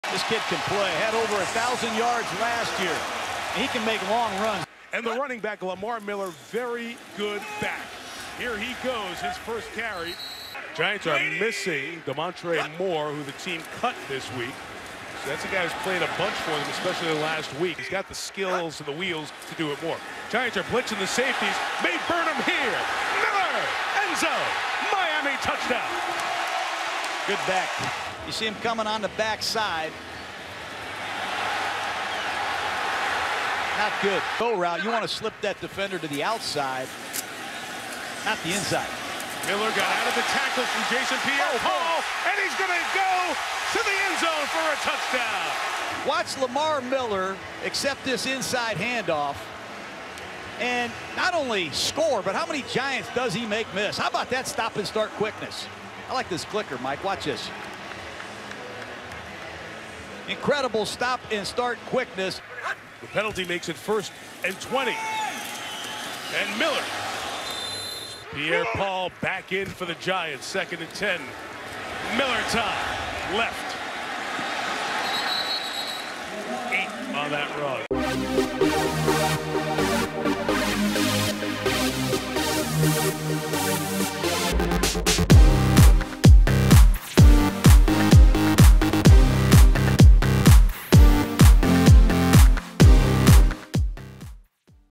Lamar Miller Highlights (Week 14) | NFL
Check out Week 14 highlights of the Miami Dolphins RB Lamar Miller taking on the New York Giants.